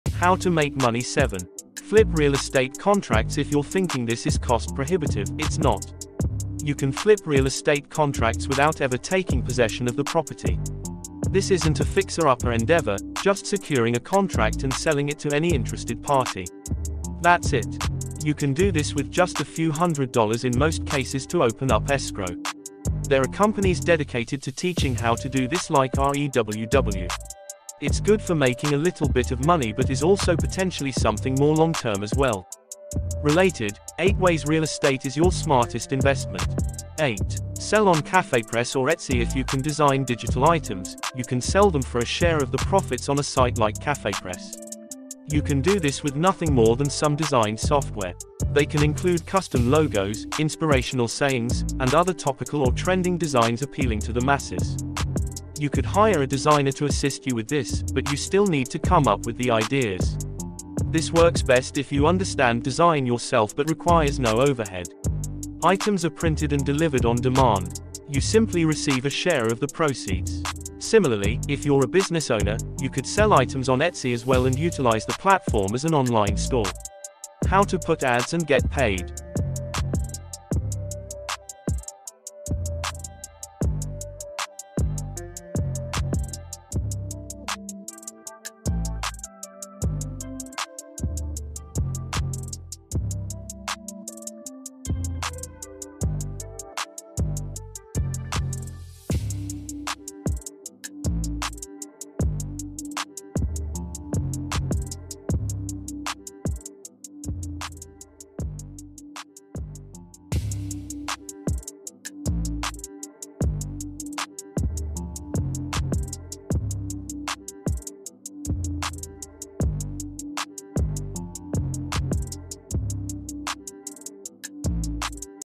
How to set up Google Ads on your YouTube video
How To Put ADS on your YouTube video and GET PAID part 2

How to put ads on your YouTube video? Can I get paid from YouTube ? How to Put ads and get paid ?
In this YouTube video you will get al information, how to put ads on your YouTube video and get paid.